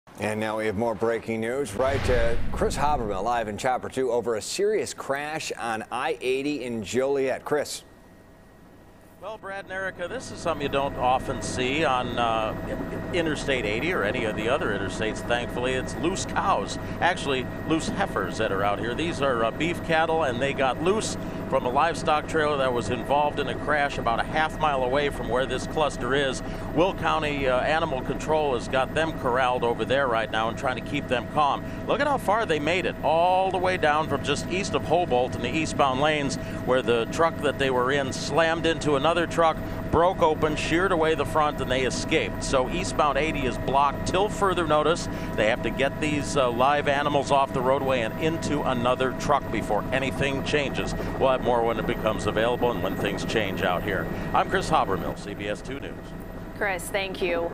I-80 shut down in Joliet after cattle hauler crashes
Several cows ended up in the road, and some did not survive the crash. Kris Habermehl reports from Chopper 2.